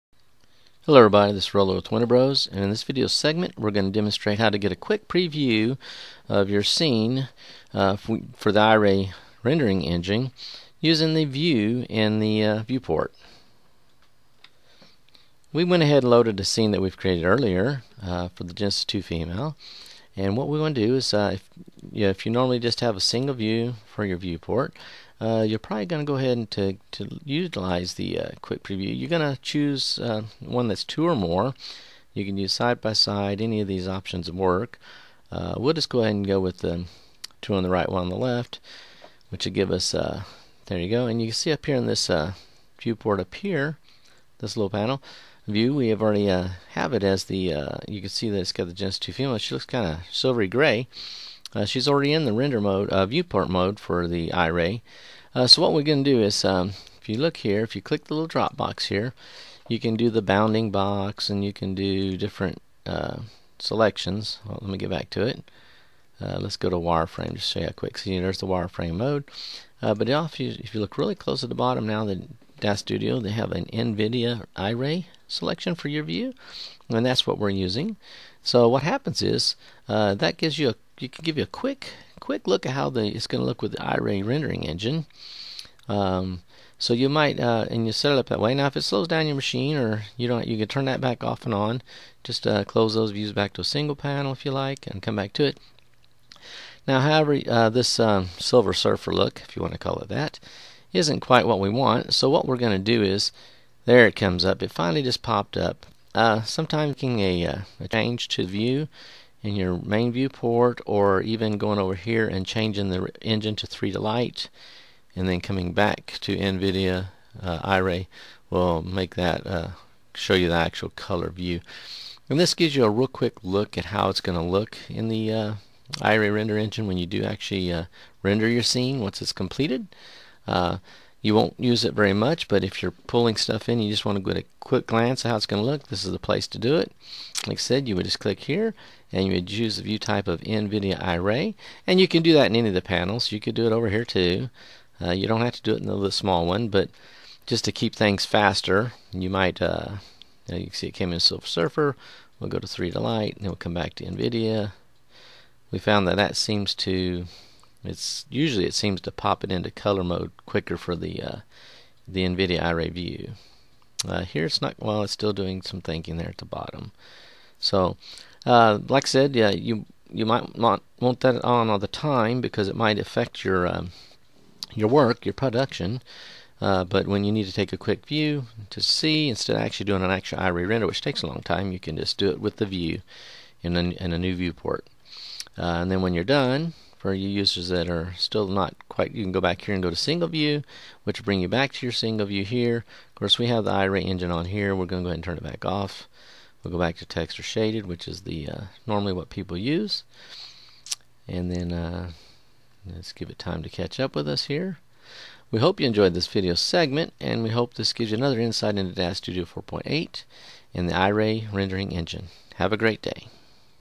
DAZ Studio: Quick Preview with Iray View
WOW!  Using a separate viewport in NVIDIA Iray mode can give you a quick and easy preview of what your scene will look like when rendered using the Iray Rendering Engine.